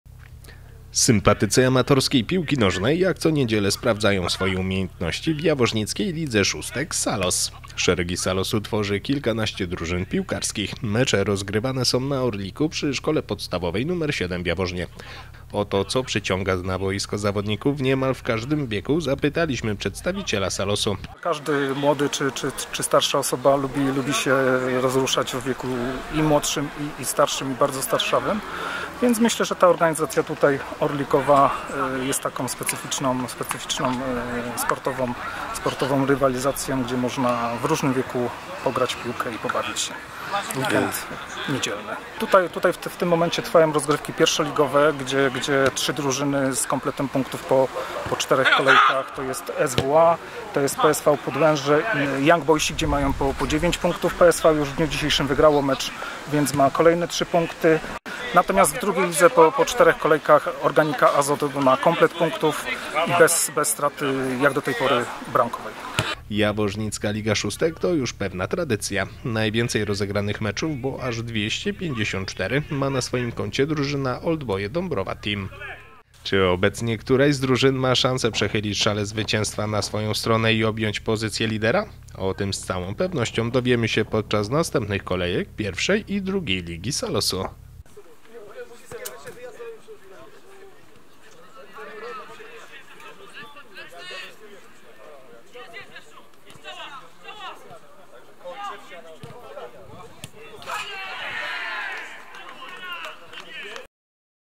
Rozgrywki JLS SALOS
Sympatycy amatorskiej piłki nożnej jak co niedzielę sprawdzają swoje umiejętności w Jaworznickiej Lidze Szóstek SALOS.
Szeregi SALOS-u tworzy kilkanaście drużyn piłkarskich. Mecze rozgrywane są na Orliku przy Szkole Podstawowej nr 7 w Jaworznie. O to, co przyciąga na boisko zawodników niemal w każdym wieku, zapytaliśmy przedstawiciela SALOS-u.